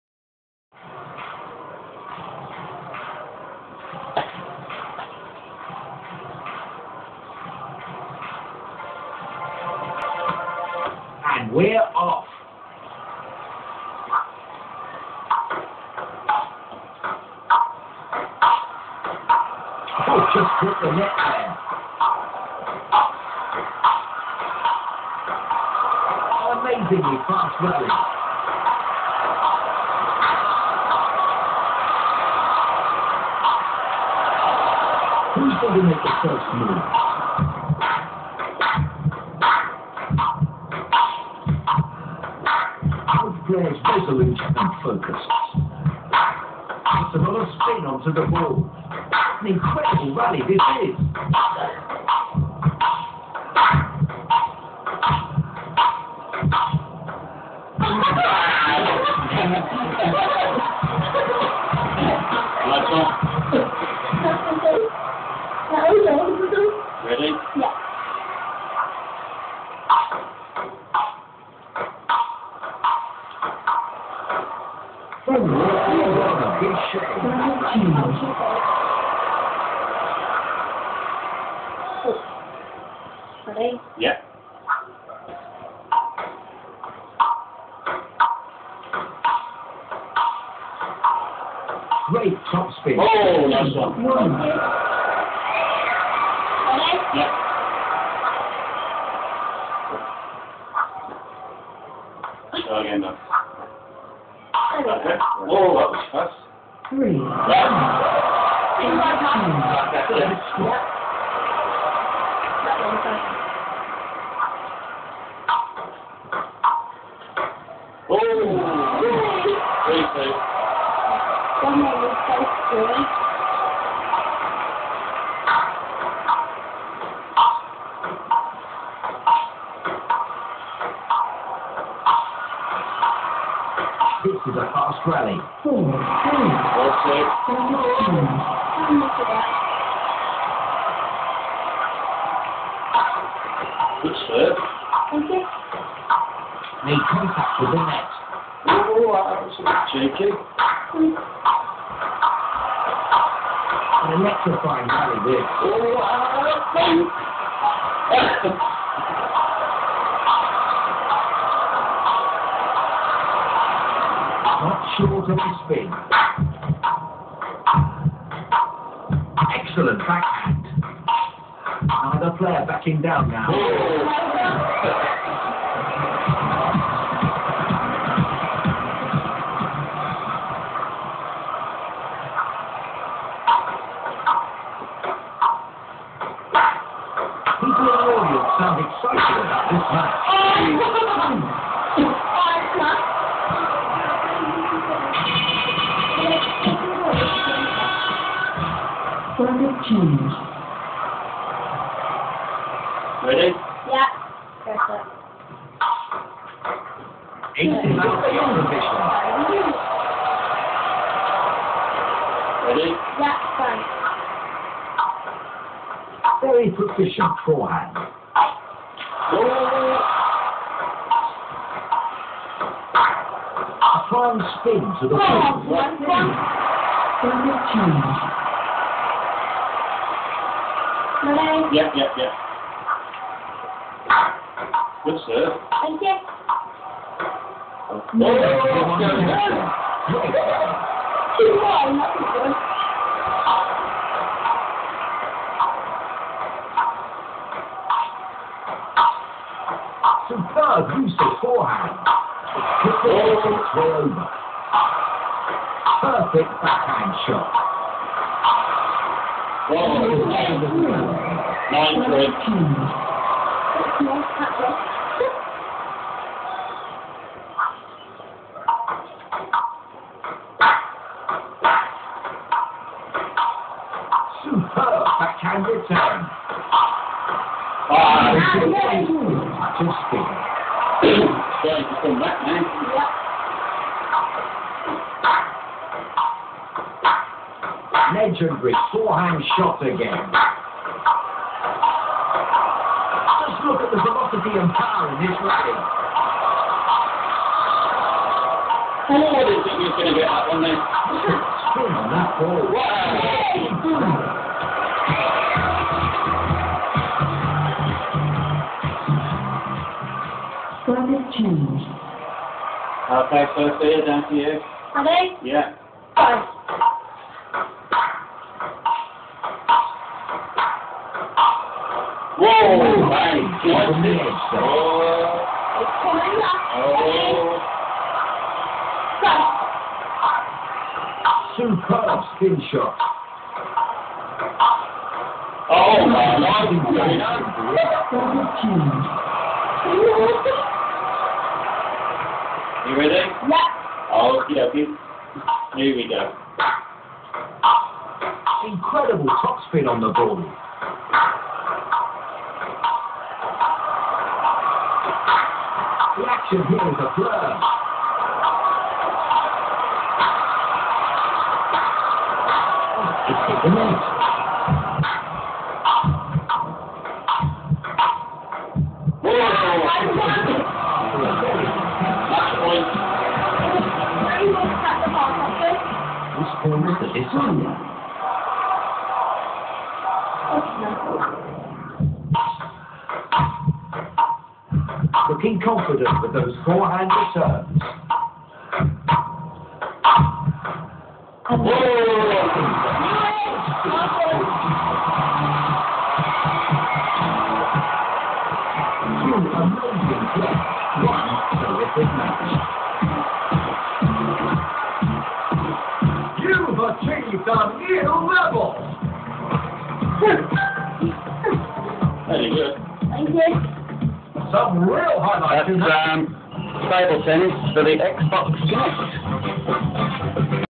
xbox Kinect
myself and my daughter sophia in an epic battle of table tennis